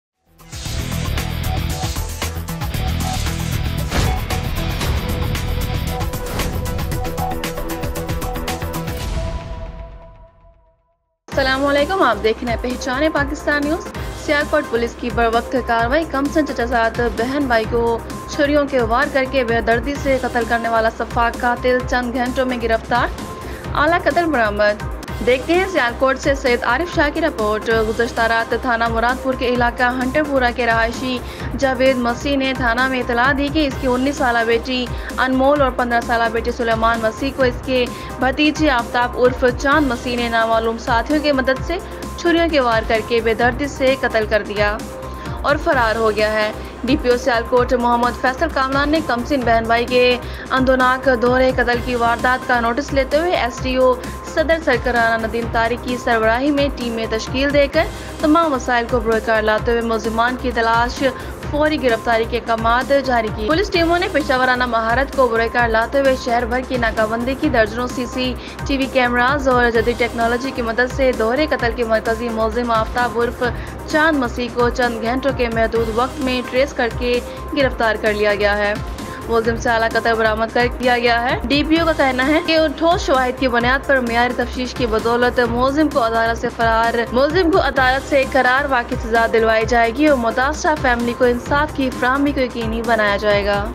سیالکوٹ کمسن چچازاد بہن بھائی کو چھریوں کے وار کر کے بیدردی سے قتل کرنے والا سفاک قاتل گرفتار
پہچان پاکستان نیوز سیالکوٹ سے سید عارف شاہ کی رپورٹ

سیالکوٹ پولیس کی بروقت کاروائی کمسن چچازاد  بہن بھائی کو چھریوں کے وار کر کے بیدردی سے قتل کرنے والا سفاک قاتل چند گھنٹوں میں گرفتار آلہ قتل برآمد۔
 گزشتہ رات  تھانہ مراد پور کے  علاقہ ہنٹر پورہ کے رہائشی جاوید مسیح نے  تھانہ میں اطلاع دی کے اس  کی  19 سالہ بیٹی انمول اور 15 سالہ  بیٹے سلیمان مسیح کو اس کے بھتیجے آفتاب عرف چاند مسیح نے نامعلوم ساتھیوں کی مدد سے چھریوں کے وار کر کے بیدردی سے قتل کر دیا ہے اور فرار ہو گیا ہے۔
ڈی پی او سیالکوٹ محمد فیصل کامران نے کمسن بہن  بھائی کے اندوہناک  دوہرے قتل کی واردات کا نوٹس لیتے ہوئے  ایس ڈی پی او صدر سرکل رانا ندیم طارق کی سربراہی میں ٹیمیں تشکیل دے  کر  تمام وسائل کو بروئے کار لاتے ہوے ملزمان کی تلاش و فوری گرفتاری کے احکامات جاری کئے۔
پولیس ٹیموں نے  پیشہ وارانہ مہارت کو بروئے کار لاتے ہوئے شہر بھر کی ناکہ بندی کی درجنوں سی سی ٹی وی کیمراز اور جدید ٹیکنالوجی کی مدد سے  دوہرے قتل کے مرکزی ملزم   آفتاب عرف چاند مسیح کو  چند گھنٹوں کے محدود وقت  میں ٹریس  کر کے گرفتار کر لیا ہے. ملزم سے آلہ قتل برآمد کر لیا گیا ہے
ڈی پی او کا کہنا ہے کہ ٹھوس شواہد کی بنیاد پر معیاری تفتیش کی بدولت ملزم کو عدالت سے قرار واقعی سزا دلوائی جائے  گی اور متاثرہ فیملی کو انصاف کی فراہمی کو یقینی بنایا جائے گا۔